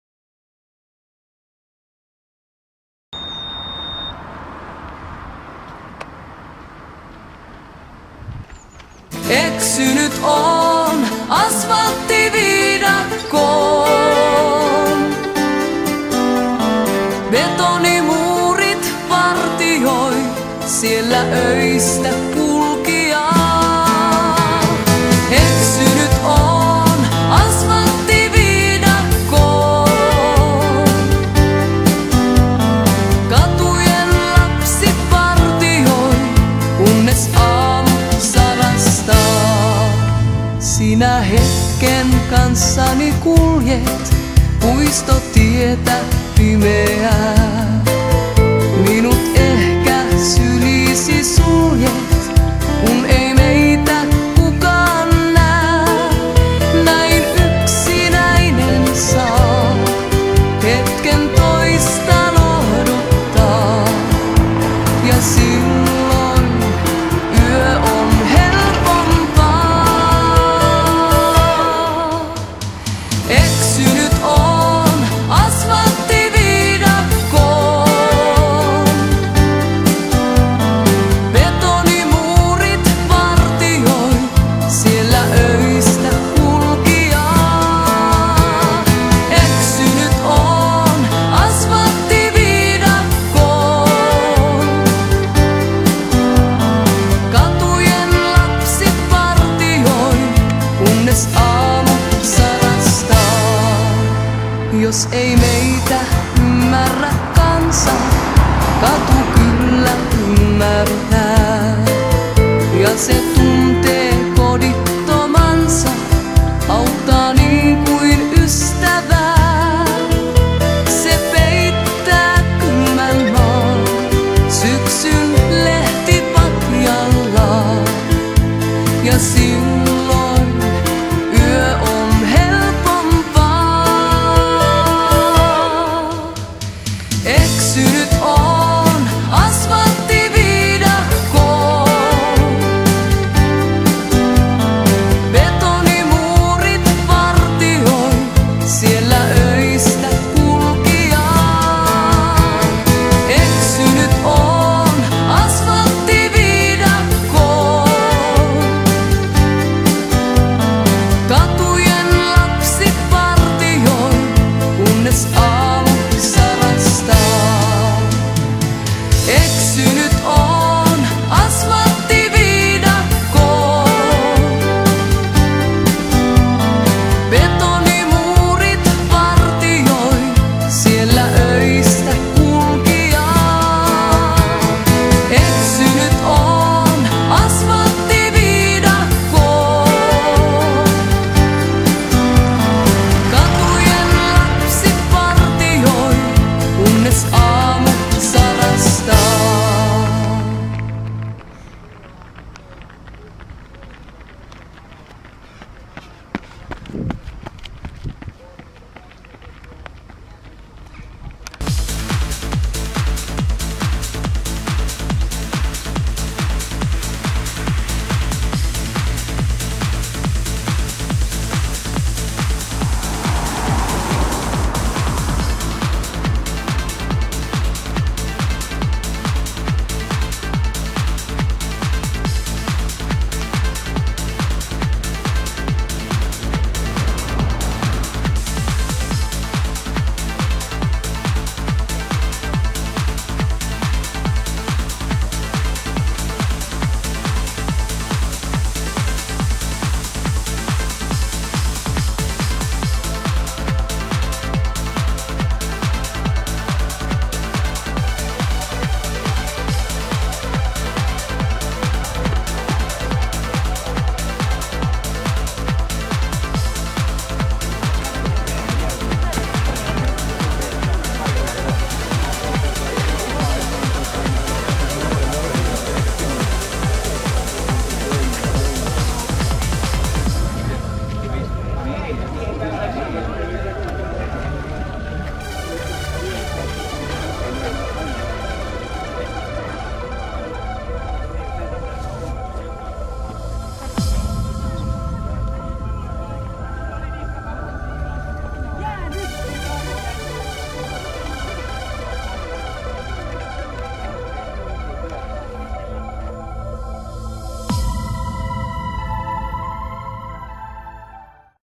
AM SPRINTTI PAIMIOSSA
Varsinais-Suomen sprint orienteering competitions in Paimio.